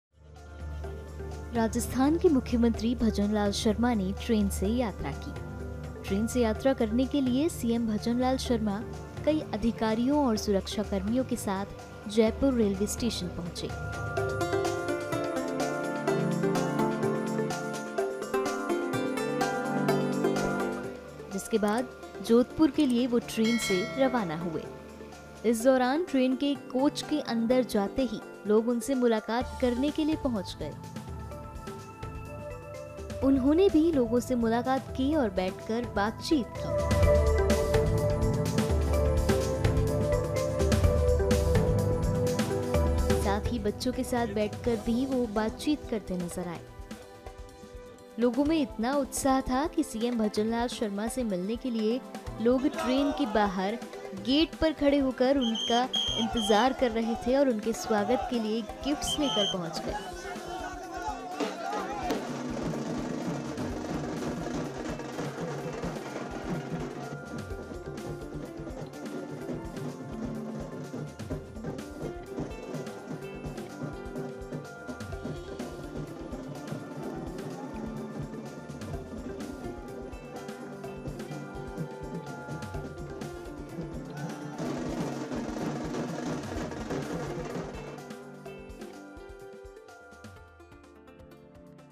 Rajasthan CM Bhajanlal Sharma ने Train से किया Travel, Passengers से की मुलाकात | दैनिक भास्कर हिंदी
राजस्थान के मुख्यमंत्री भजनलाल शर्मा ने ट्रेन से यात्रा की। ट्रेन से यात्रा करने के लिए सीएम भजनलाल शर्मा कई अधिकारियों और सुरक्षाकर्मियों के साथ जयपुर रेलवे स्टेशन पहुंचे। जिसके बाद जोधपुर के लिए वो ट्रेन से रवाना हुए। इस दौरान ट्रेन के कोच में अंदर जाते ही, लोग उनसे मुलाकात करने के लिए पहुंच गए। उन्होंने भी लोगों से मुलाकात की और बैठकर बात-चीत की। साथ ही बच्चों के साथ बैठकर भी वो बात-चीत करते नजर आए। लोगों में इतना उत्साह था, कि सीएम भजनलाल शर्मा से मिलने के लिए लोग ट्रेन के गेट पर खड़े हो गए और उनके लिए गिफ्ट्स लेकर आए।